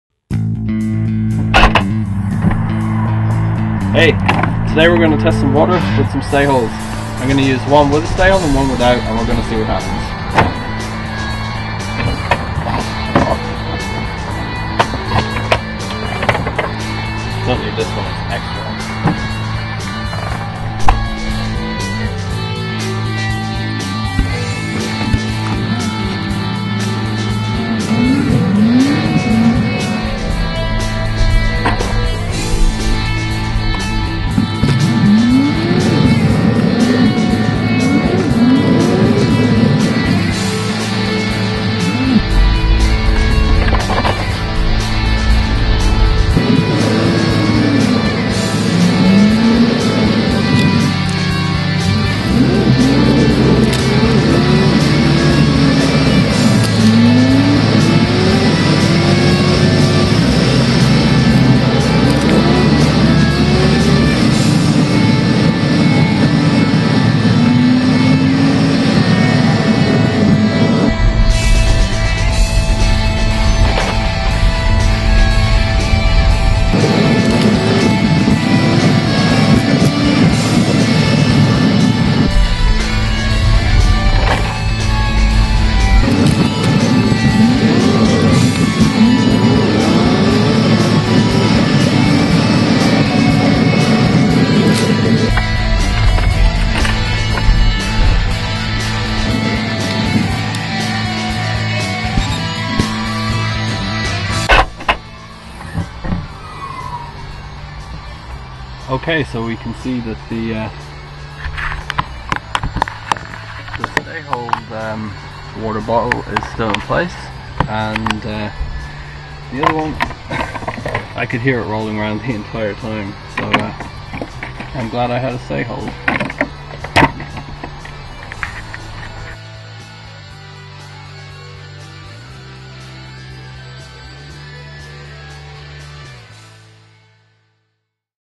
Stayhold™ - Episode 2 - Big Water Bottles tested with Stayhold Cargo Companions
Let's test some big bottles of water, one with a Stayhold and one without.
This does sounds like I have a Formula One engine in my Focus but this was about a 10 minute car journey so some of the footage is sped up otherwise there would be long sections when there is not much happening when we are on straight sections of road.